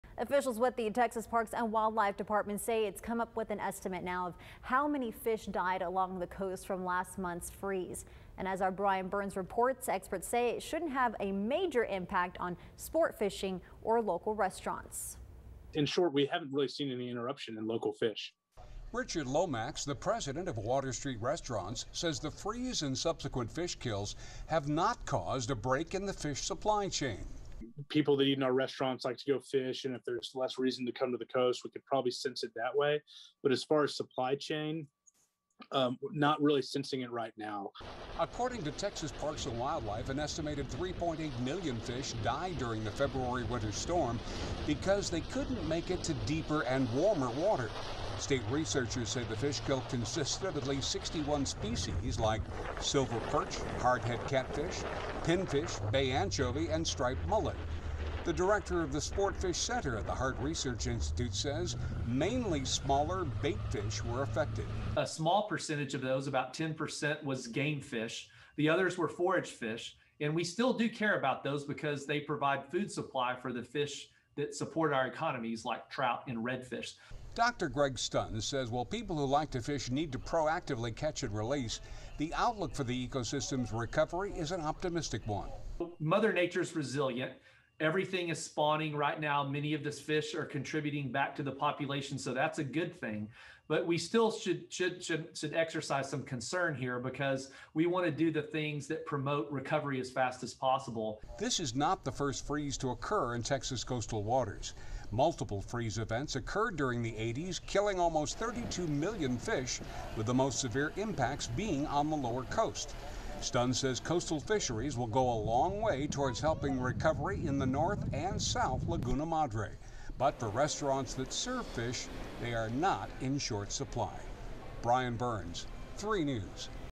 Coastal Bend fish supply not affected after millions of fish washed up dead on the the coast
"In short we haven't really seen any interruption in local fish," Said Richard Lomax, President of Water Street Restaurants.